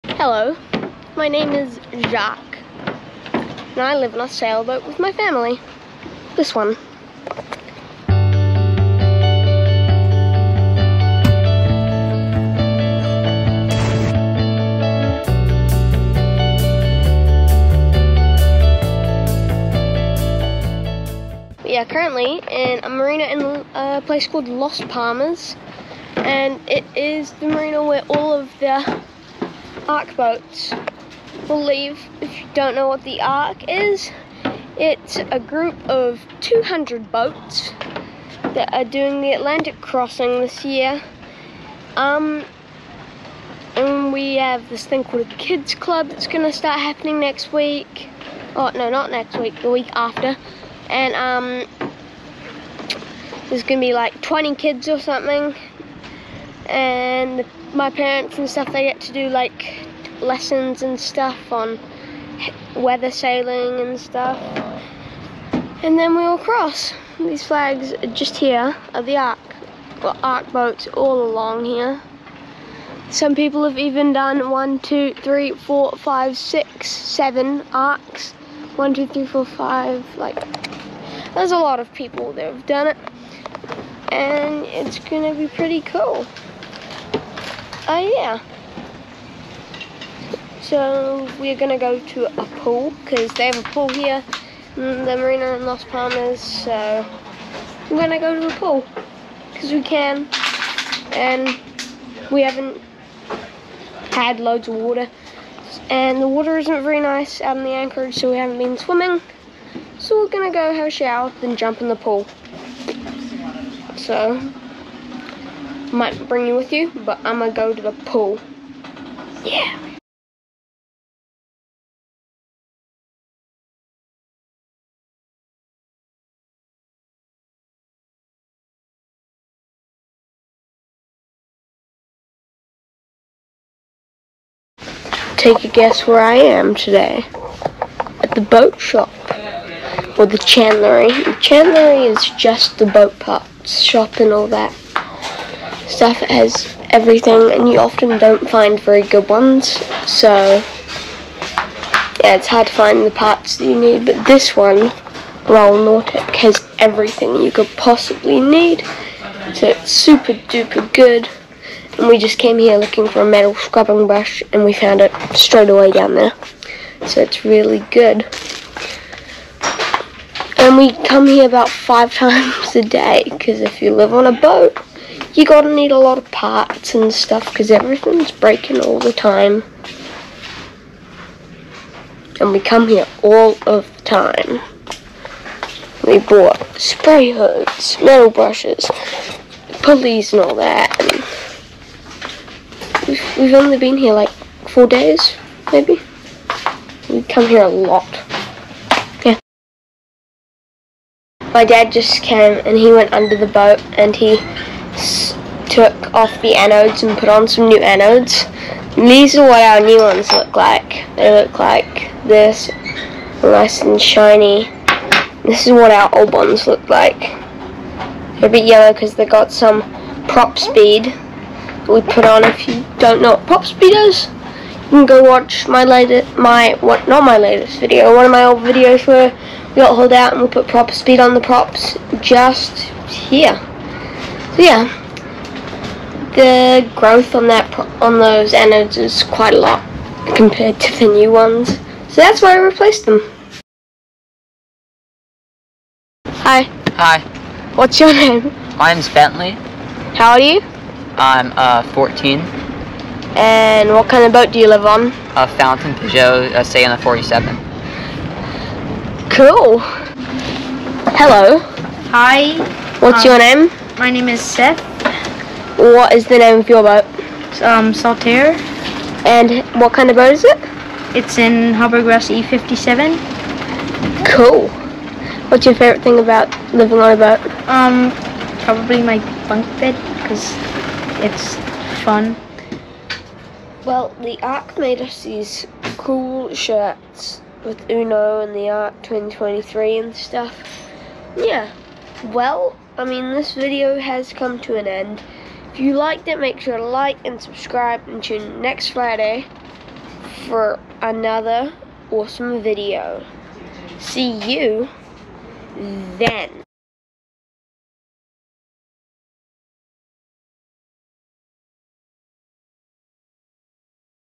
New friends for the Atlantic Crossing (Ep 24)
in this video I I talk about the ARC Atlantic crossing group introduce my friends